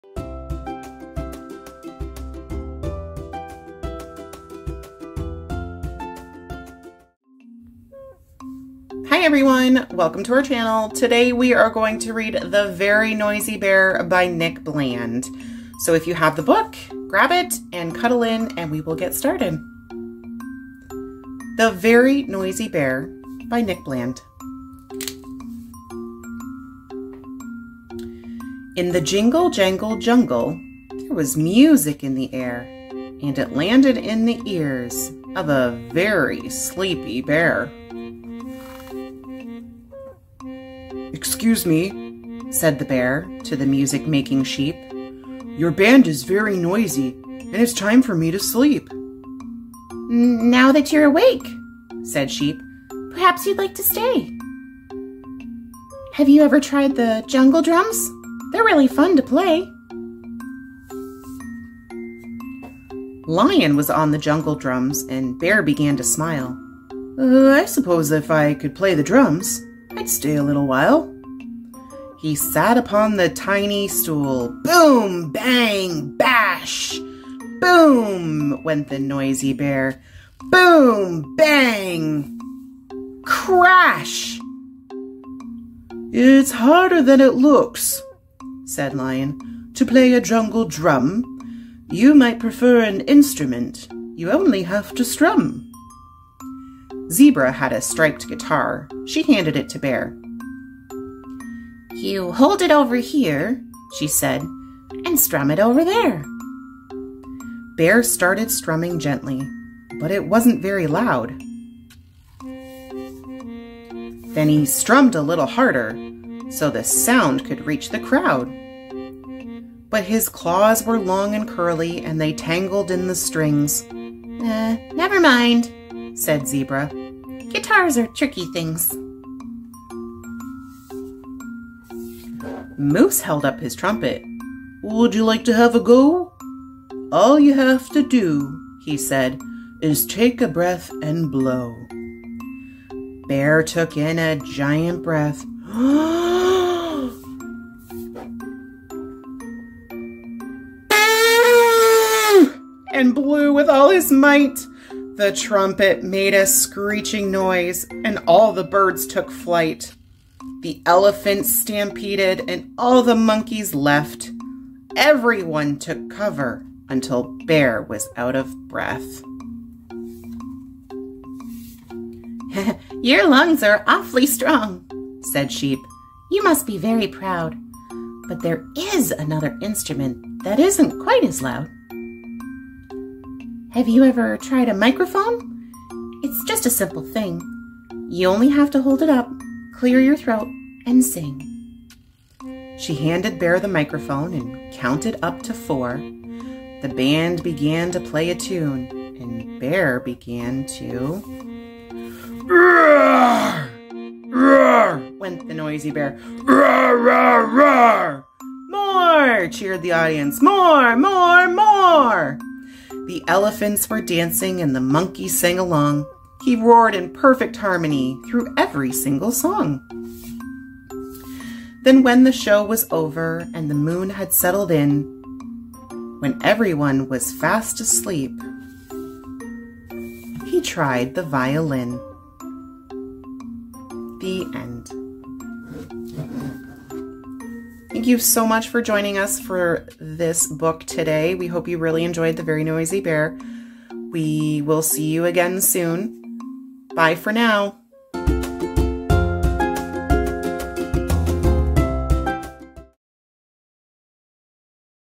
The Very Noisy Bear by Nick Bland - read-a-long
Join us as we read The Very Noisy Bear by Nick Bland